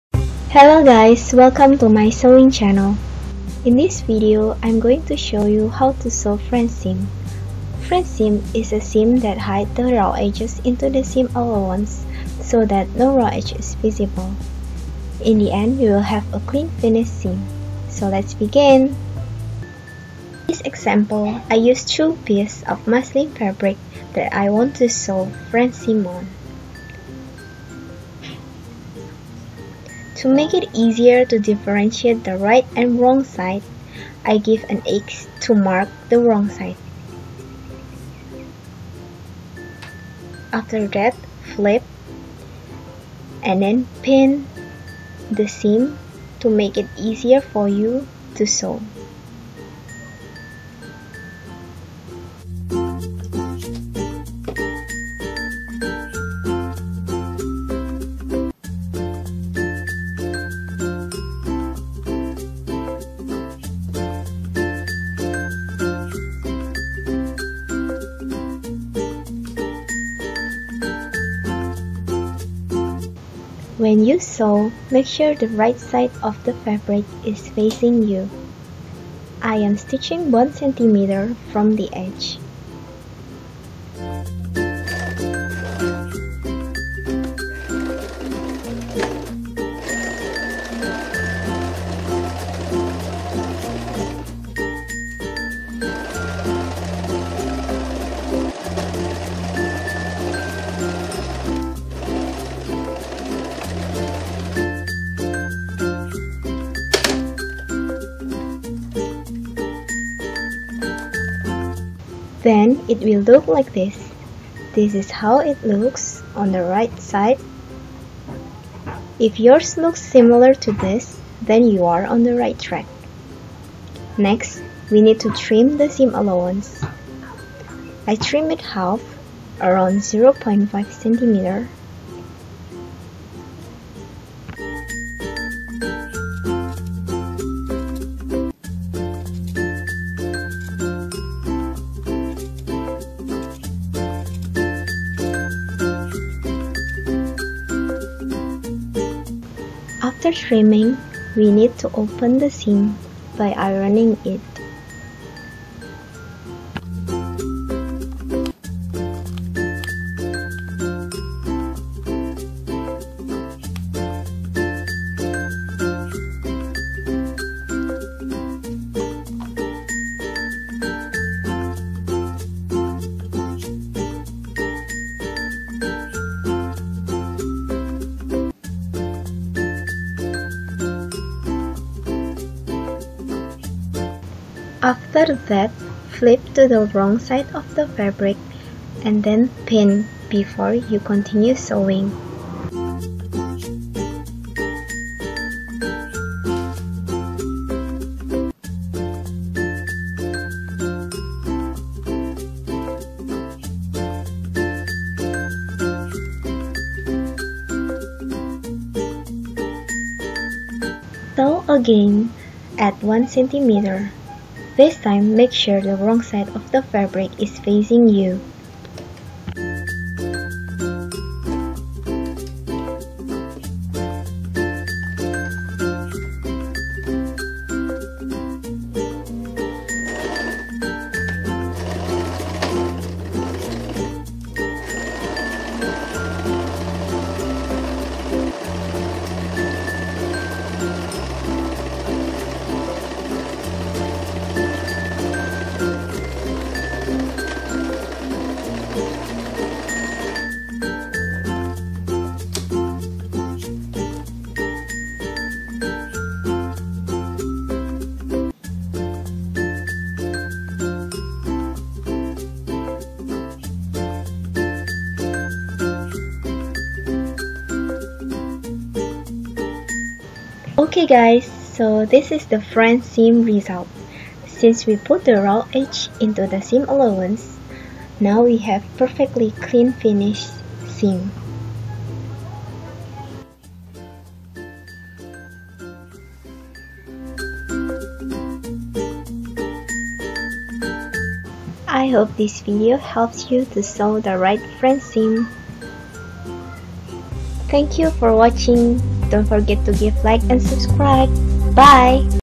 [Basic] How to Sew French Seam
Sharing to you on how to sew french seam.
We often see french seam used on lightweight or sheer fabrics (tulle, chiffon, organza, etc), to provide a nice clean-finished seam without overlocking. Have you ever tried it before? What do you think?

Machine used: Singer Tradition 2282 (portable)

Thank you and see you on my next video :)